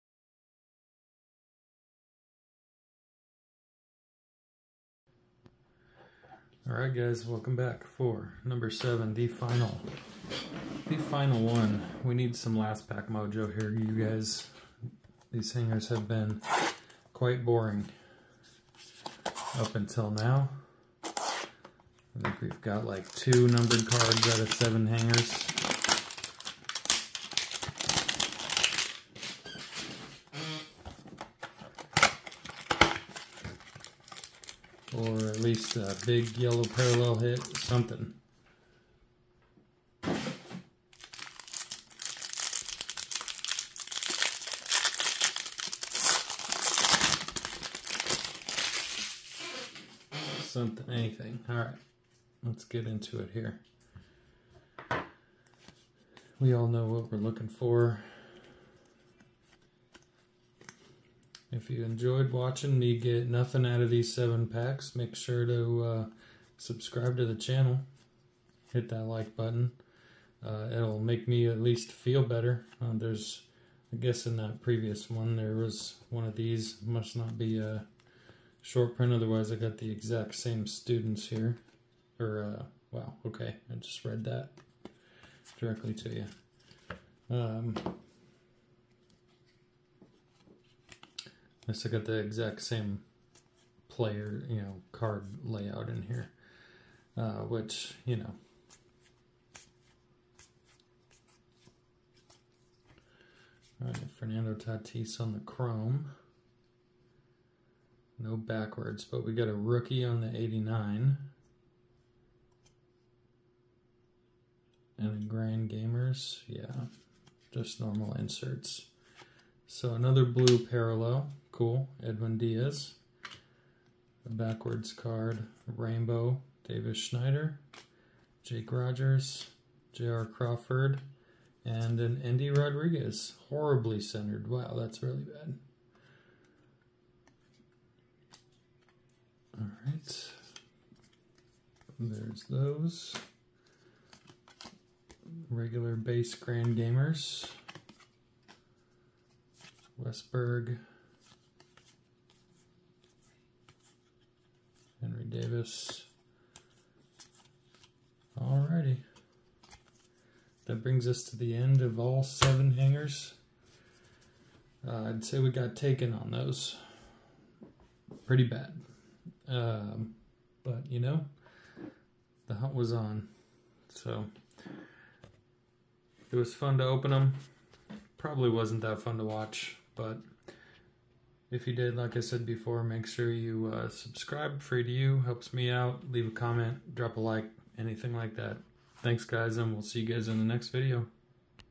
THE LAST HANGER... 2024 Topps Series 1 Hanger 7 of 7
We did it! we made it through all 7! it was a wild ride but we made it! check out the ebay store: